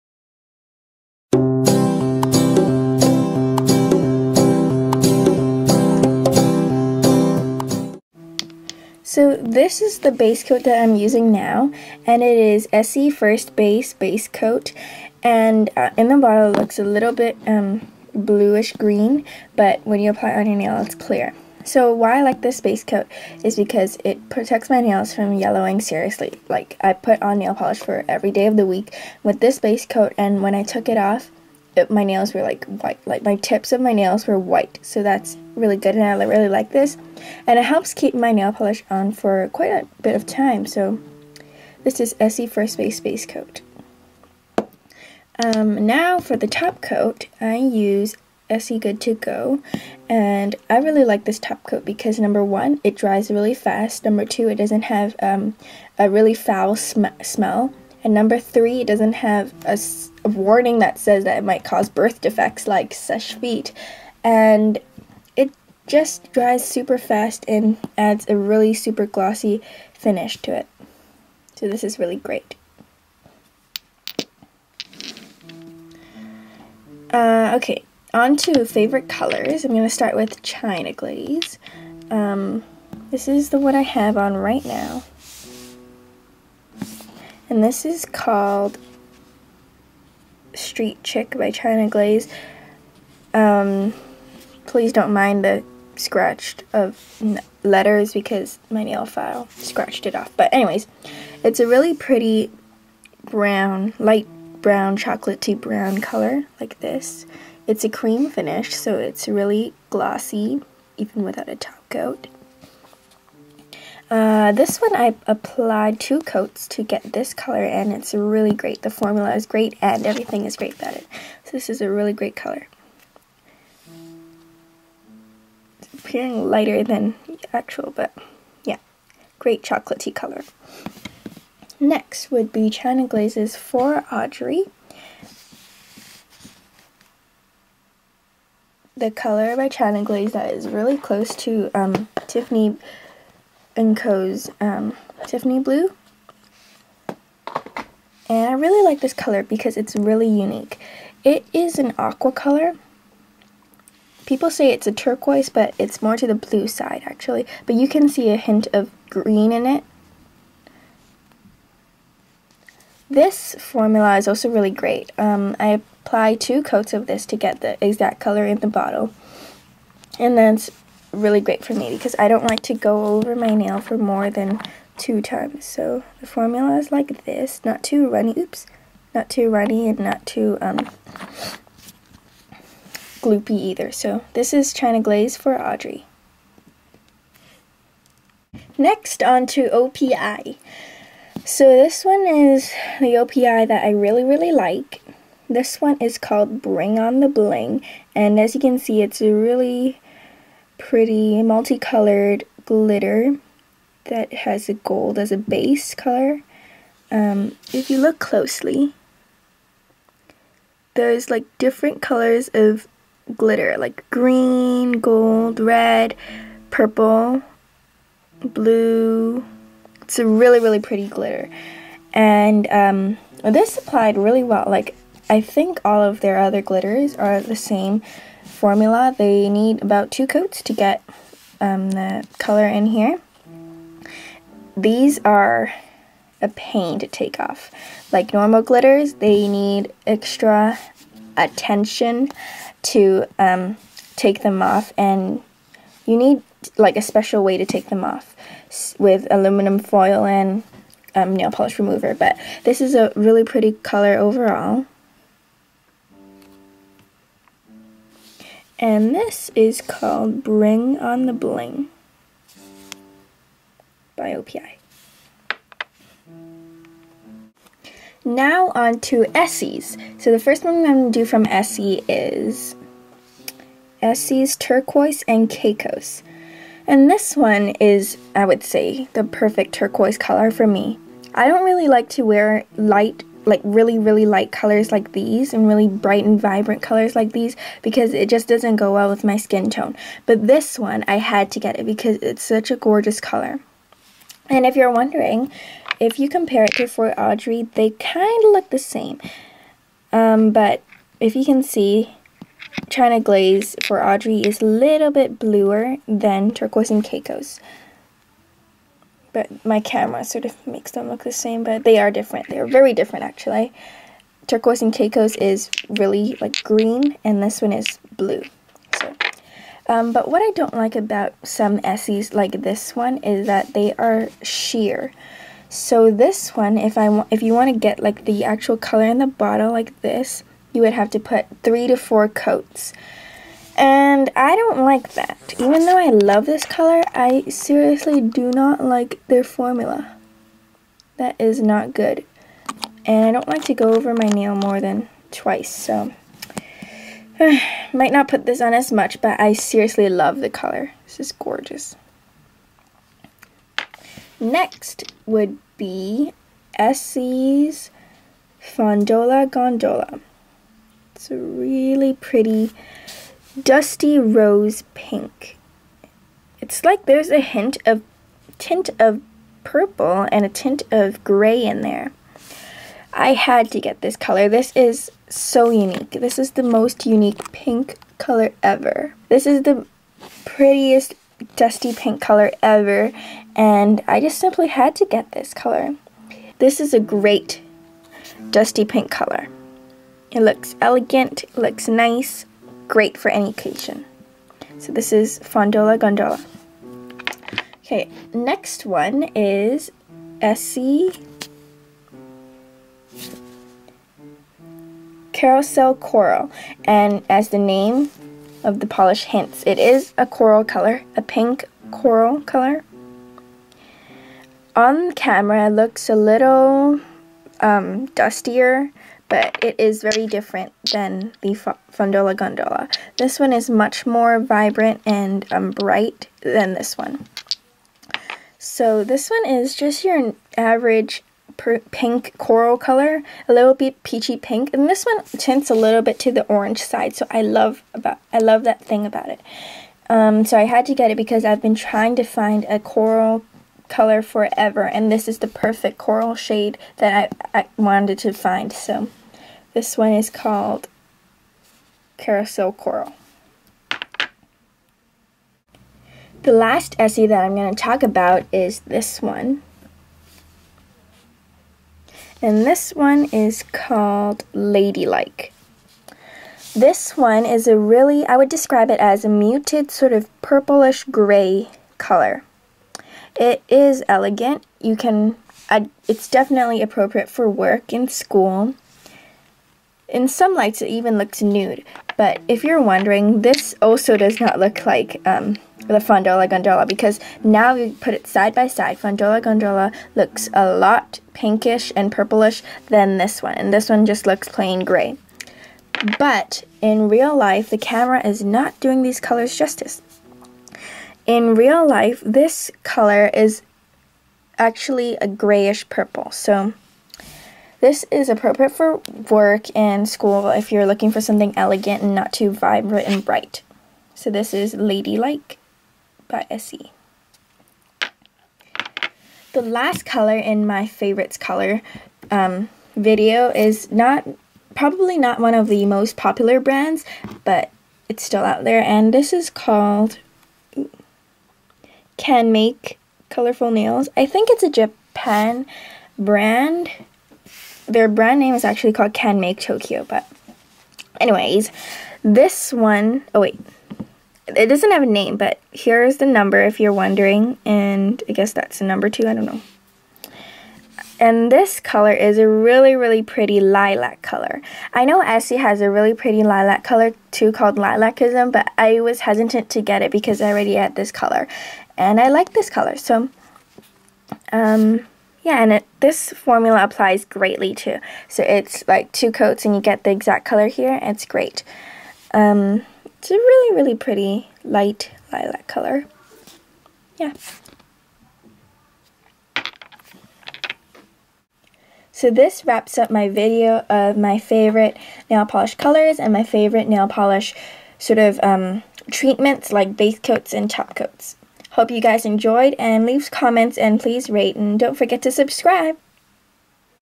Favorite Nail Products
products mentioned:
Essie - first base base coat, good to go! topcoat, fondola gondola, carousel coral, lady like, turquoise and caicos
OPI- bring on the bling
ChG- Street Chic, for audrey
CanMake- #22